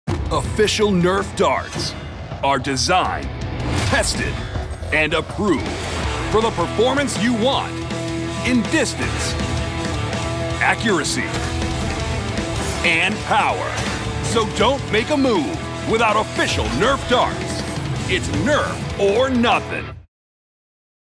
NERF - 'N' Strike Elite Darts' Official Spot
Official Nerf Darts are designed, tested and approved for distance, accuracy, and power! So don't make a move without official NERF darts!